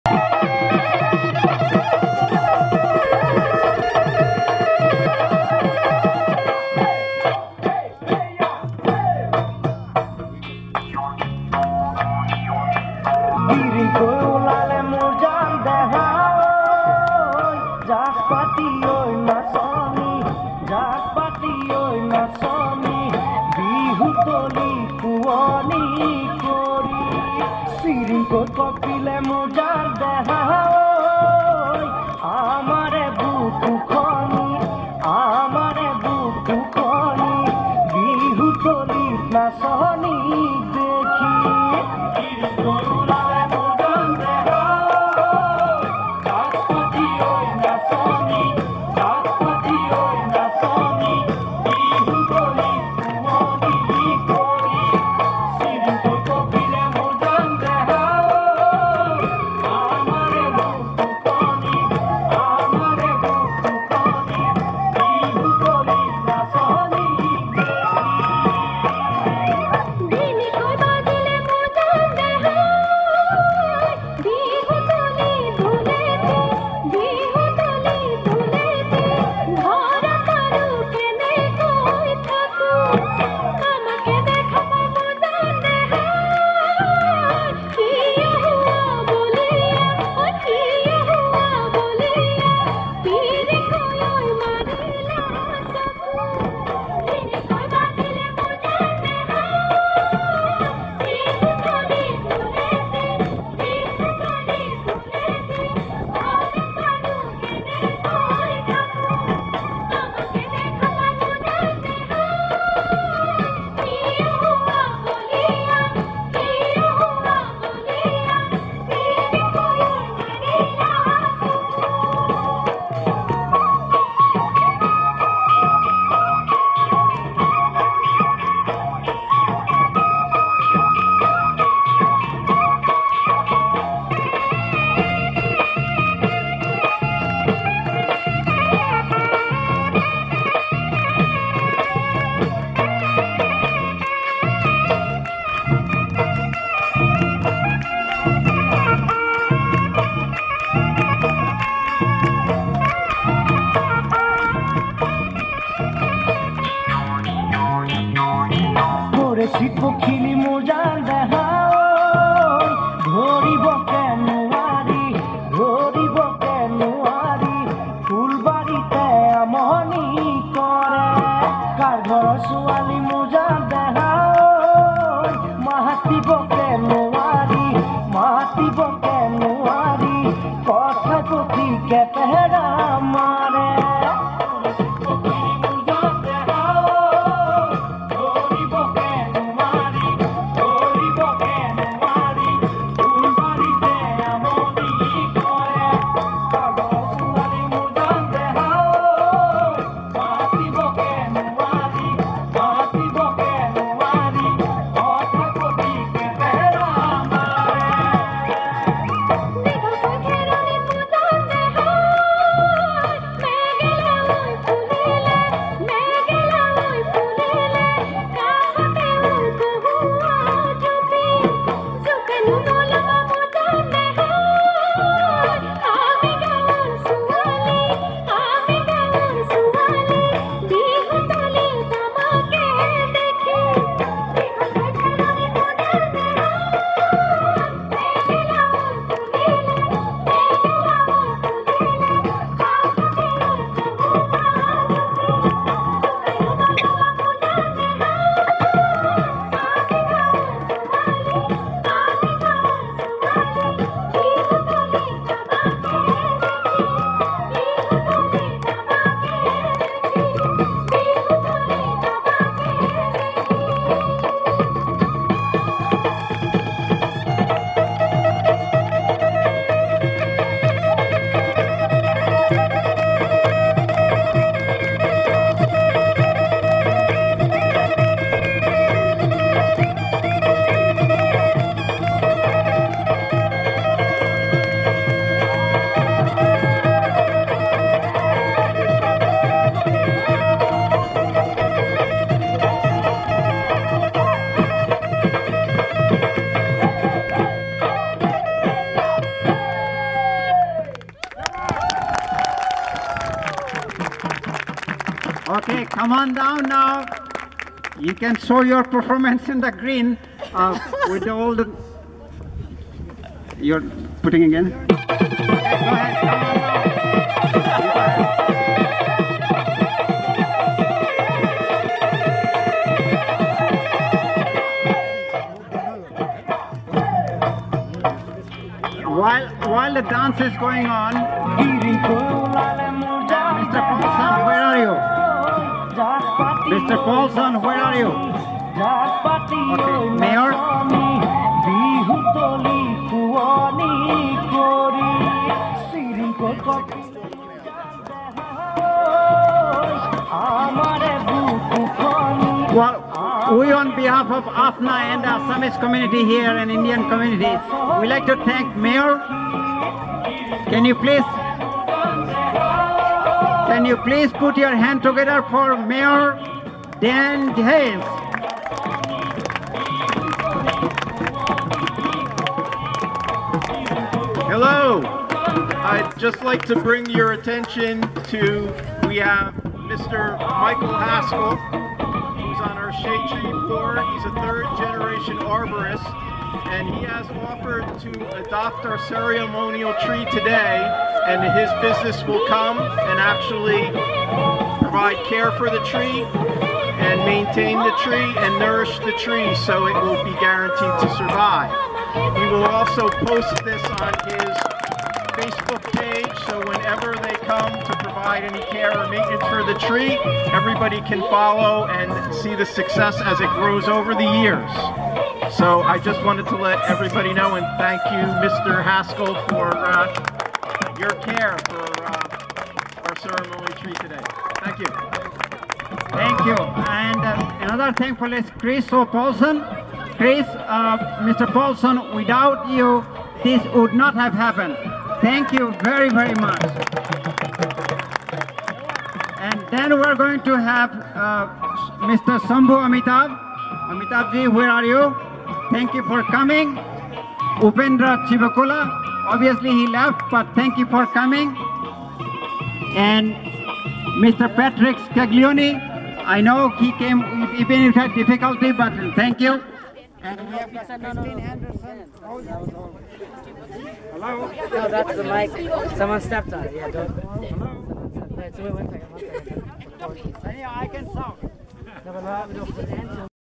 Bihu dance at Jadav Payeng Tree Planting at Chimney Rock Park, New Jersey
Bihu dance at Jadav Payeng Tree Planting at Chimney Rock Park, New Jersey. Performed by Tripti Borah, Monalisa Mozumder and Kaveri Sarma and Dr. Binoy Bordoloi, Director-AFNA Board leading from the font. October 17, 2015 at Chimney Rock Park, New Jersy, USA